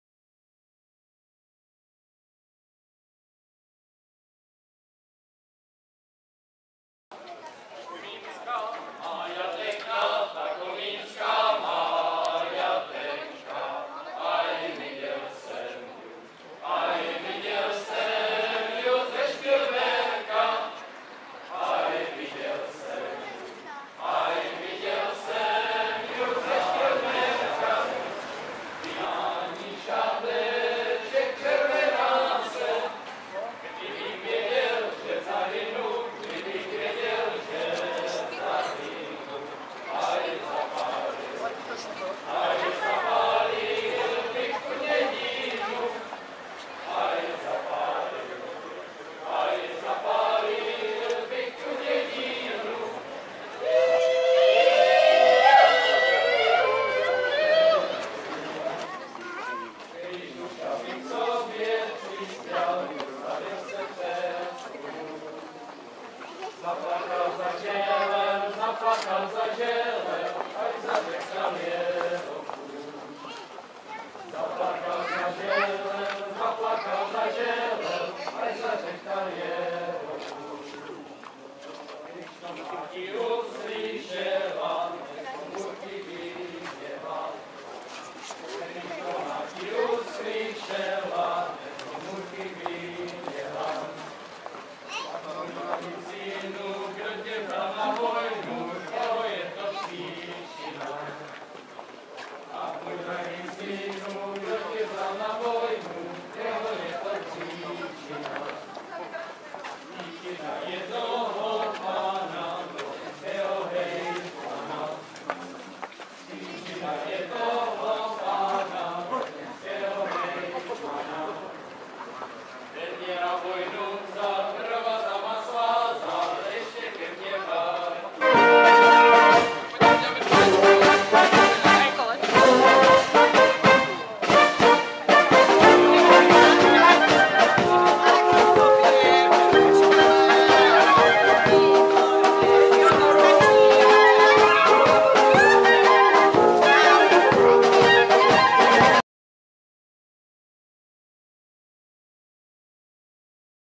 2022 - 14. srpna - Komíňáček na Hodech pod Májou
Průvod Komínských hodů zavítal pod Máju u Staré hasičky...